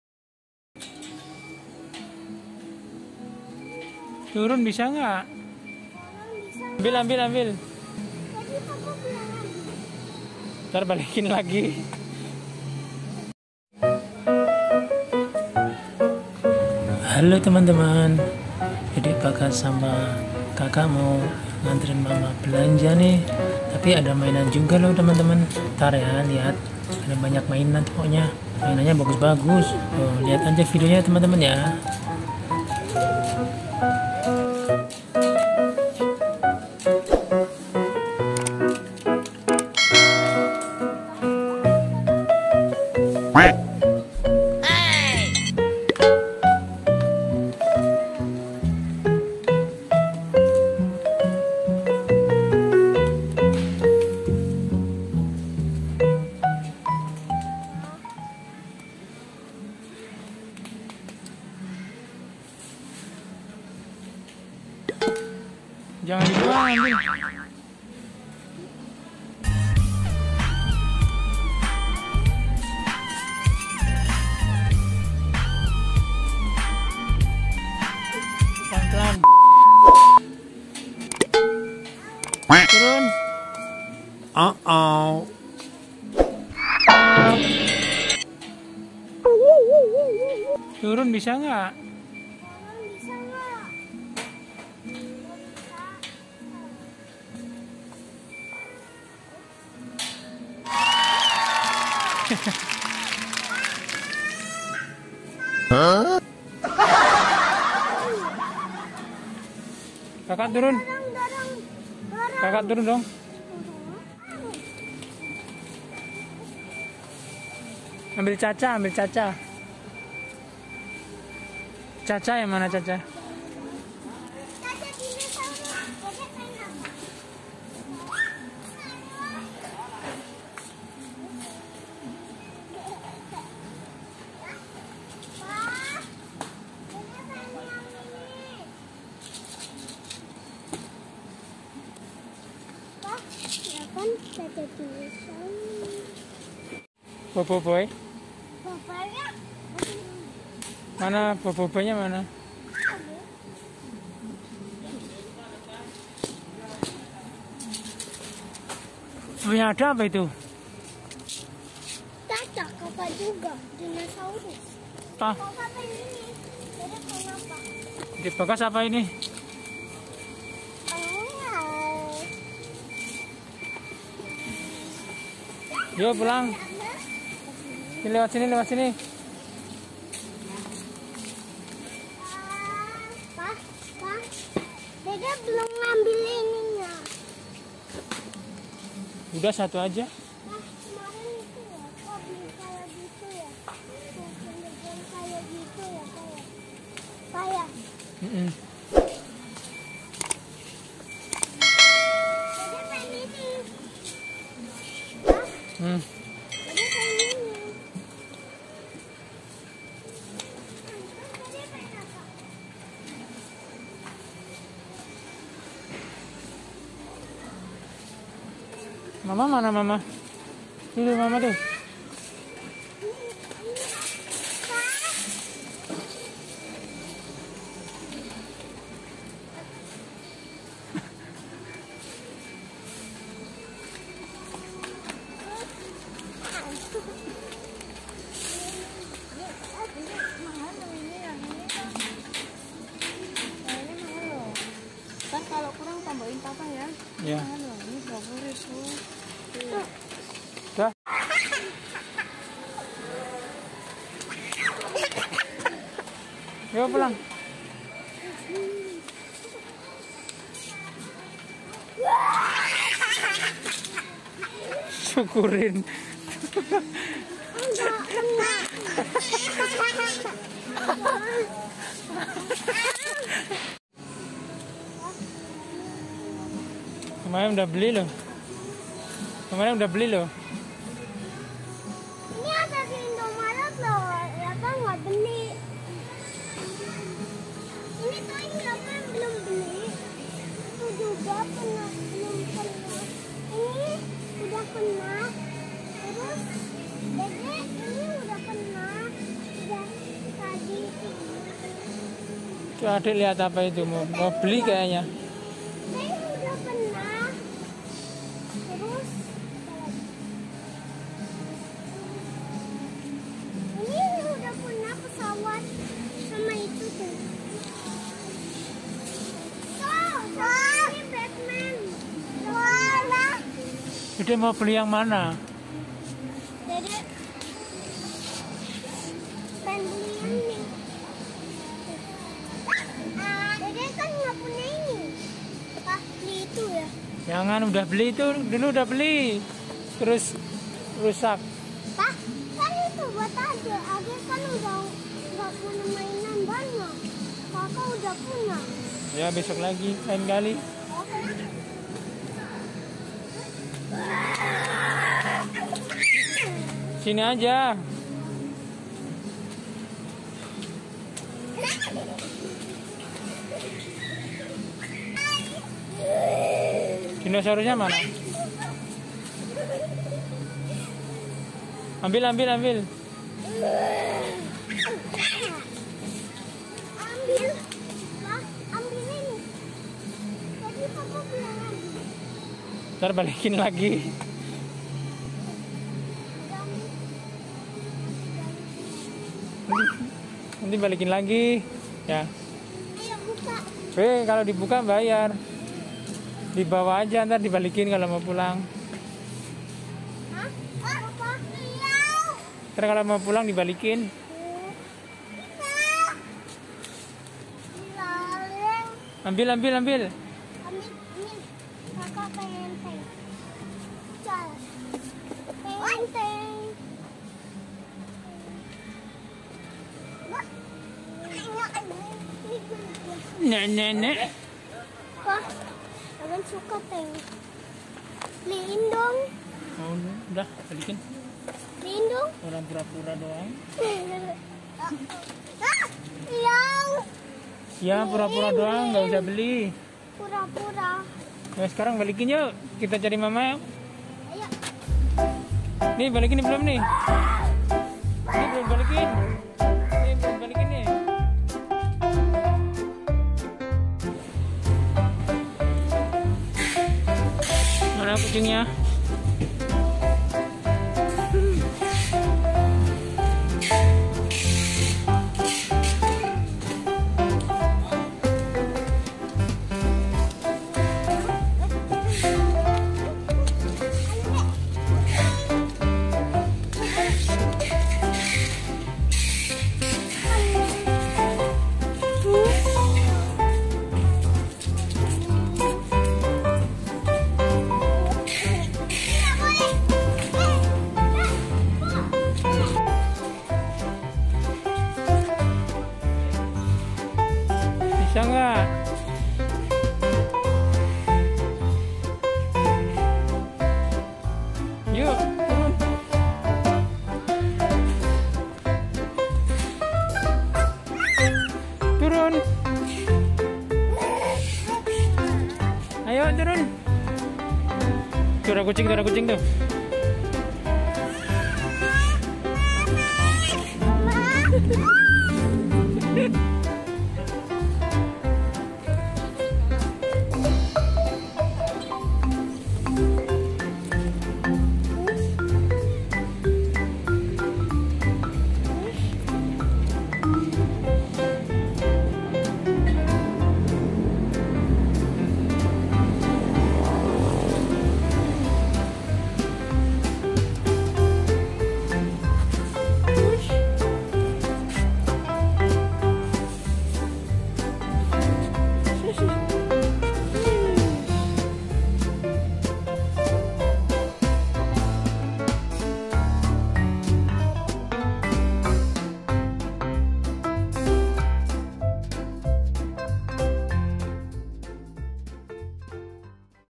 Anterin Mama Belanja bulanan nemu Banyak Mainan Anak-anak | Pulang nya dikejar Kucing lucu
Halo teman-teman kali ini Zafran sama dede Bagas mau Anterin Mama Belanja bulanan nemu Banyak Mainan Anak-anak | Pulang nya dikejar Kucing lucu

Saksikan video nya ya;Semoga terhibur

Terima kasih